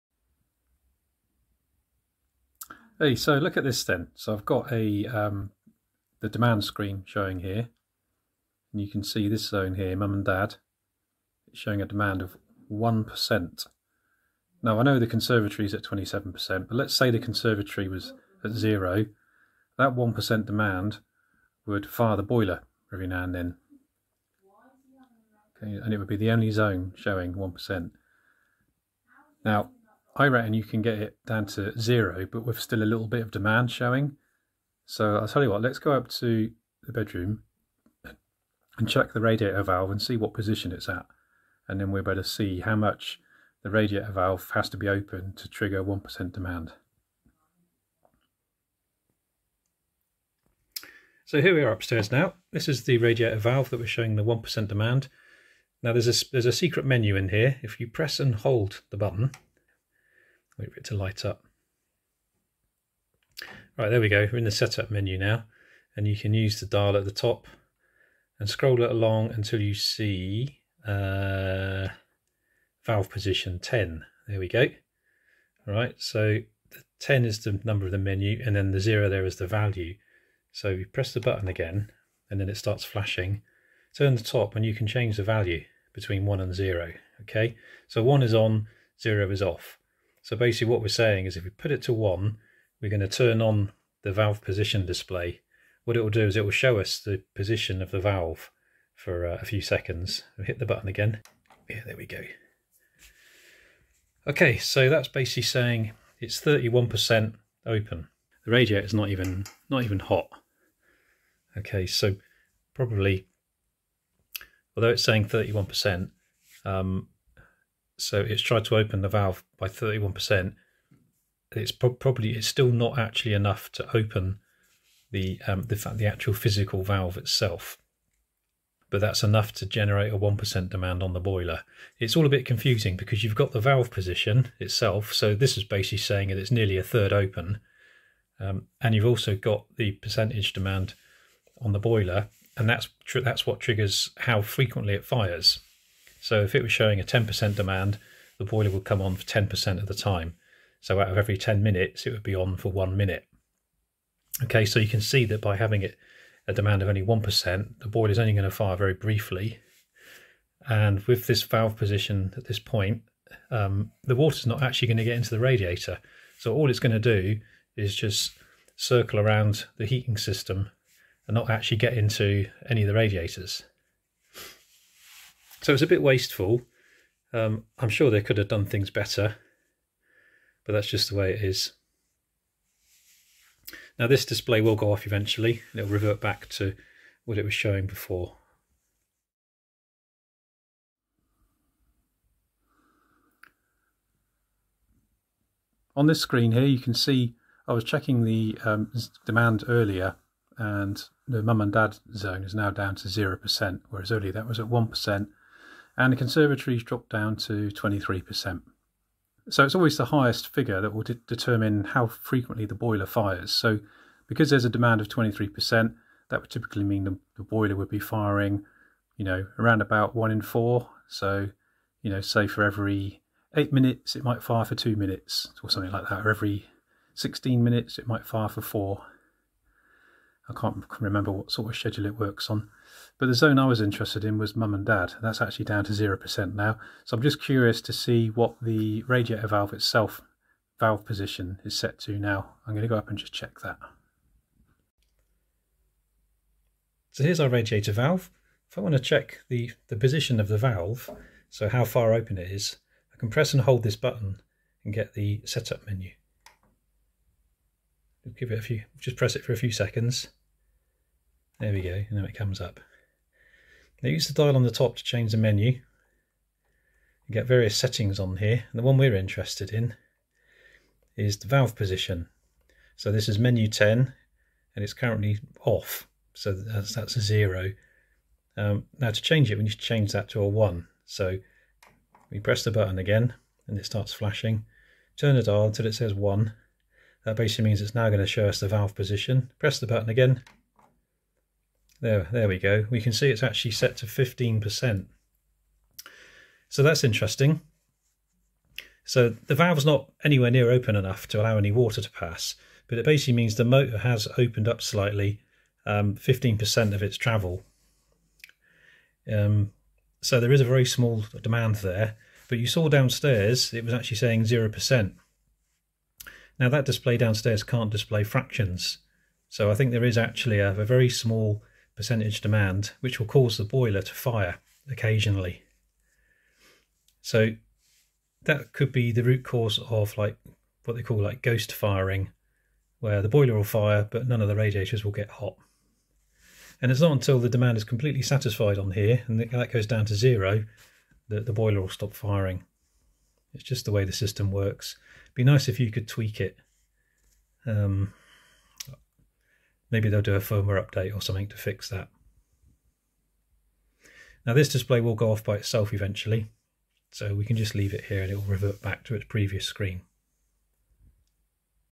Honeywell Evohome - Why is my boiler firing? Troubleshooting "phantom firing"
Sometimes the System Summary screen will display 0% demand on all zones but the boiler will still occasionally fire - in this video we check an HR92 TRV's valve position and find that even when demand is showing 0% in the System Summary screen, the TRV is still open by a small percentage which is causing a demand in the system that cannot be displayed - this will cause the boiler to fire occasionally even if the valve isn't open far enough to let water pass through into the radiator.